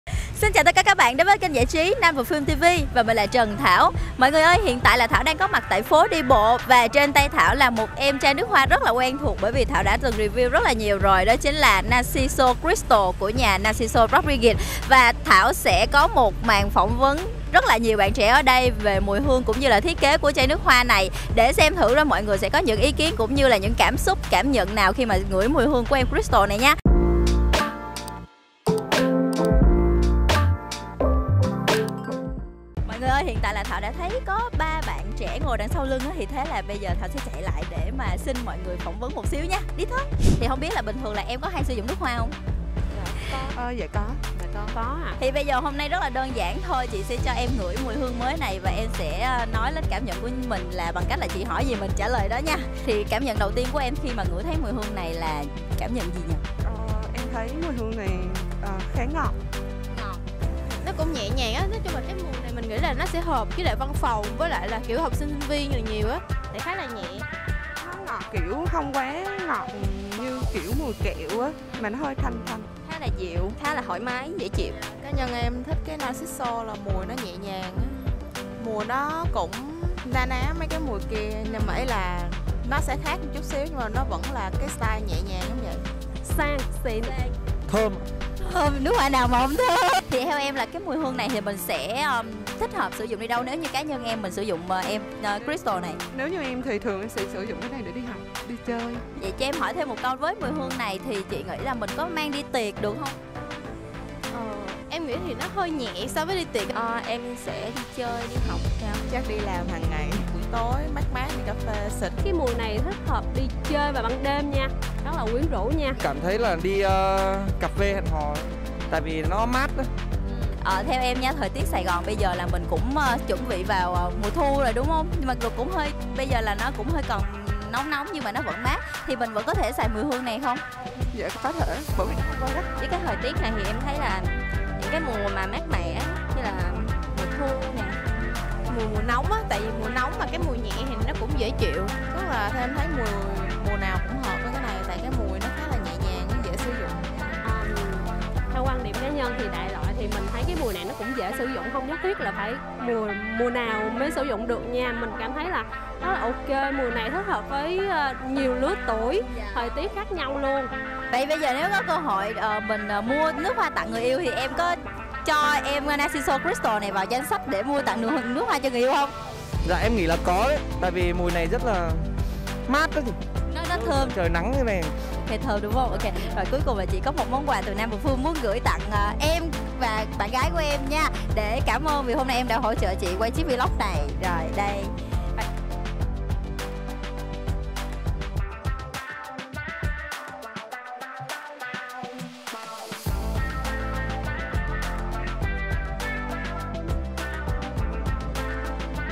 Nước hoa Narciso Cristal có được lòng mọi người? | namperfume TV
Đối với Thảo Narciso Cristal là một phiên bản nước hoa mnag mùi hương thanh tao, trẻ trung và quyến rũ nhưng không hẳn ai cũng có chung suy nghĩ và cảm nhận với Thảo. Vậy nên chúng ta hãy cùng nhau tìm hiểu xem mọi người nói gì về Narciso Cristal nhé.